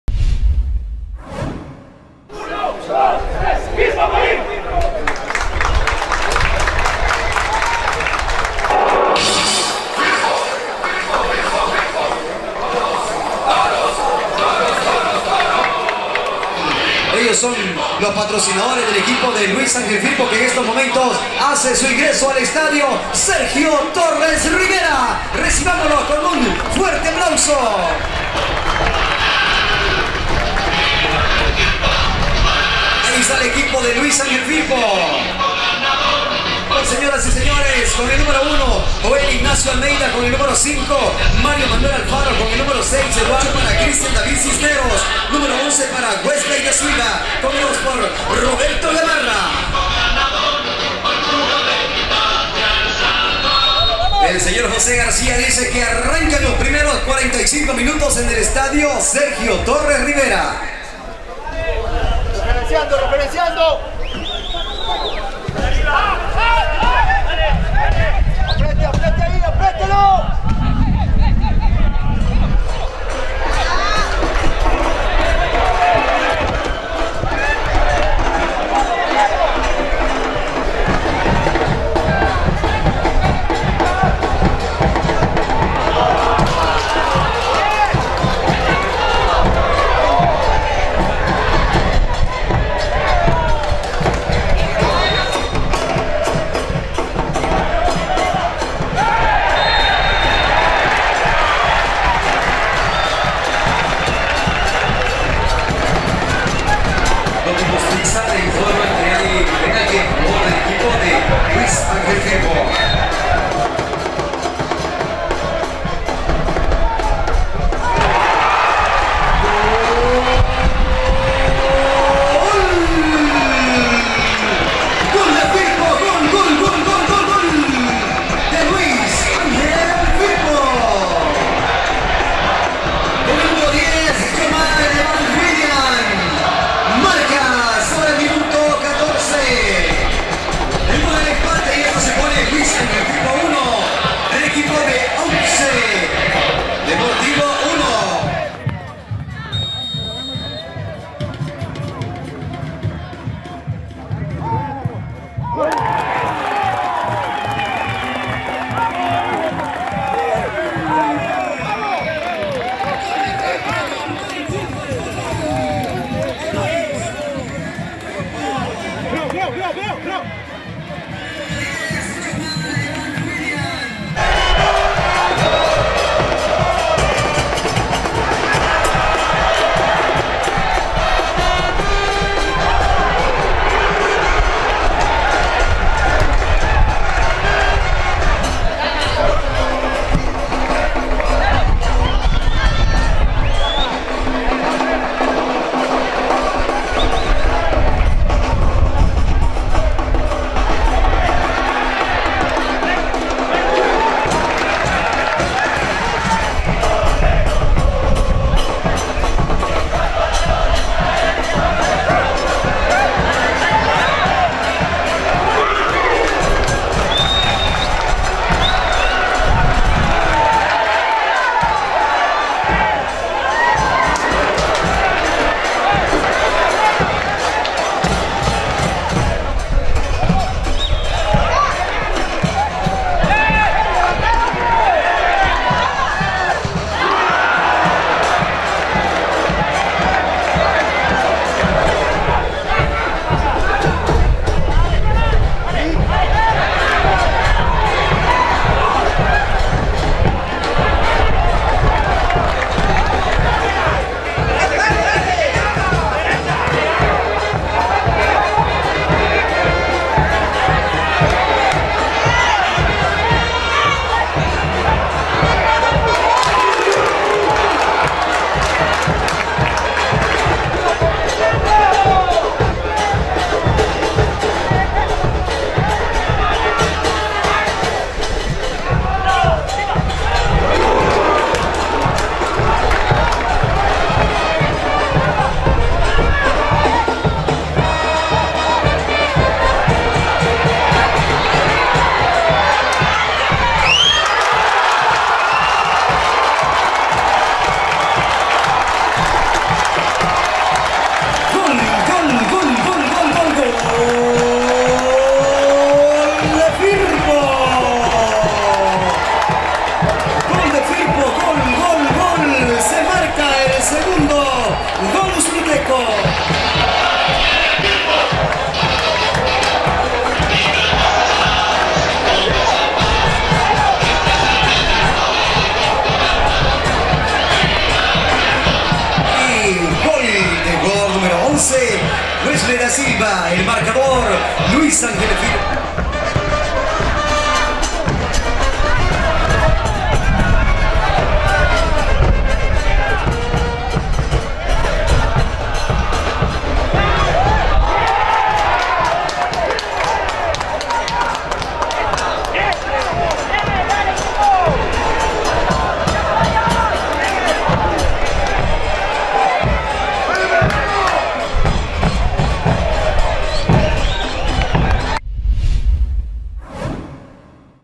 Resumen | C.D. L.Á. FIRPO 2 - 2 ONCE DEPORTIVO F.C.
Resumen | Te compartimos los mejores momentos que nos deja la fecha 6
C.D. L.Á. FIRPO 2 - 2 ONCE DEPORTIVO F.C.
Anotaciones de Jomal Williams y Wesley Tanque.